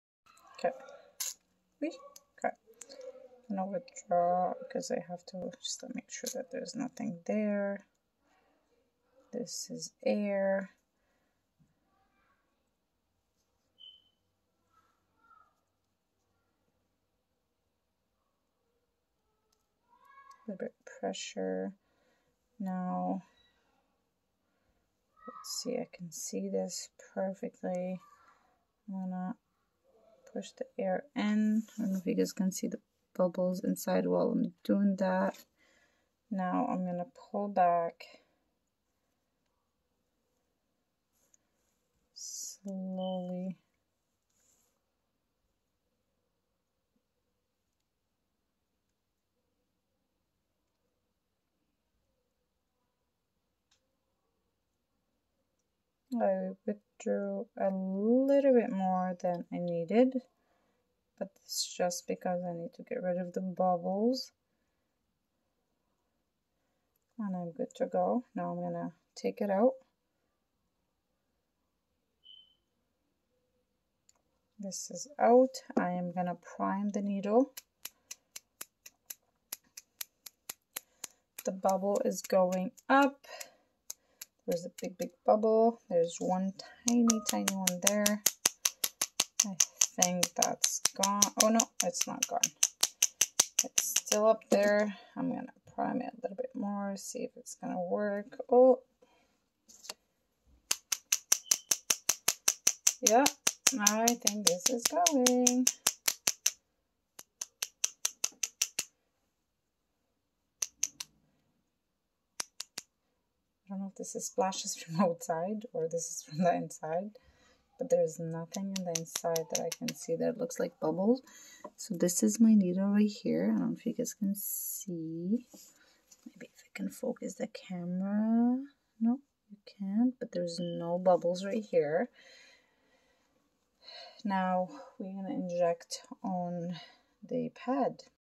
Priming needle for SC injection
As a nursing student it is essential to practice priming to remove all bubbles in the medication that we withdraw from a vial. So here i'm ONLY practicing on priming an injection for SC.
Remember:
 1. Inject air before you withdraw your medication.
2. inject from the medication not the air in the vial.
3. do not touch the plunger
4. tap the needle for priming ( no bubbles)
5. remember to recap on the table/ desk ( maintain no touch technique)